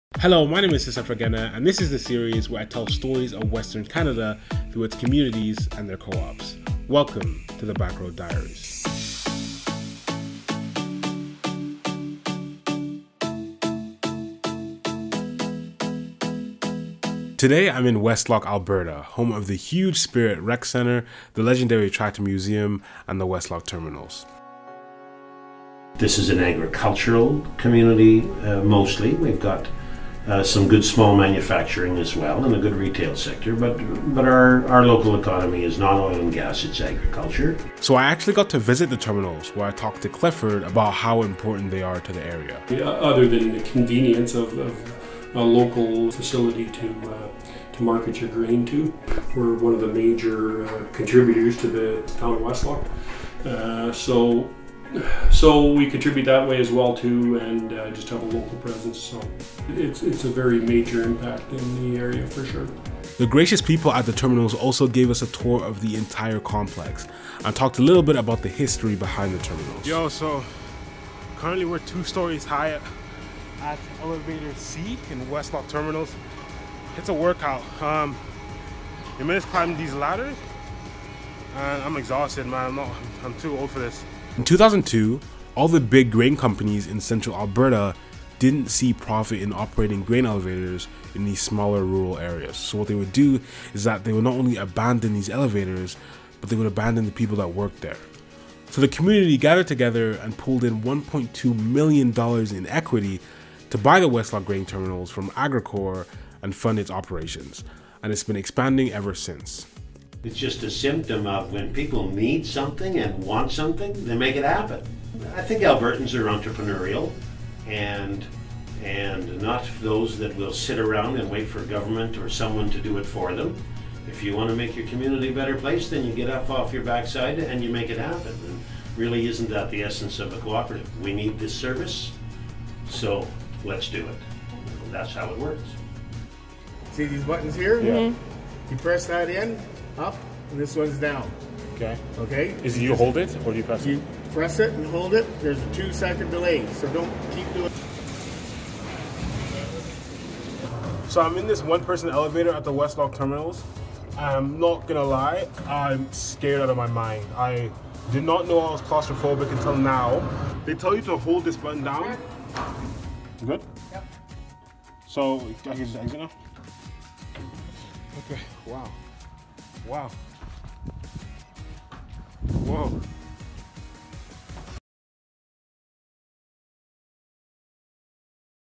The Backroad Diaries: Westlock Terminals (Westlock)
It’s hard to over emphasize the importance of quality infrastructure for robust economic activity. In rural communities, it can be especially hard to keep and maintain the things needed to attract large businesses. Westlock Terminals is the amazing story of how this community rose to the occasion.